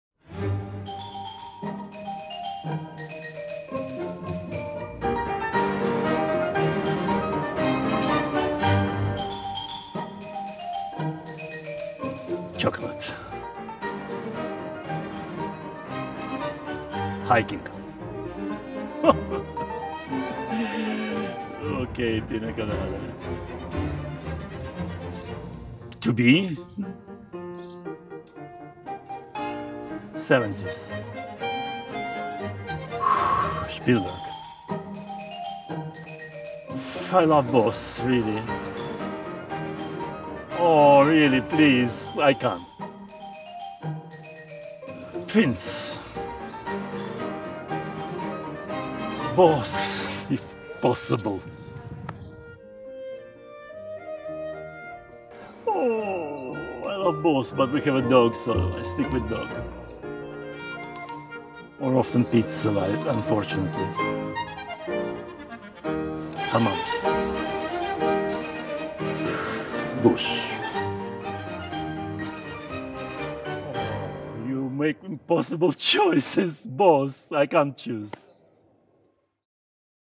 A minute with... Mischa Maisky
Chocolate or Cheese? Vodka or Caviar? Recital or Concerto? Learn more about the Cellist Mischa Maisky!

Mischa Maisky
A minute with Interview

Recorded at the Verbier Festival 2017.

© Hugo Orenga - MUSEEC/medici.tv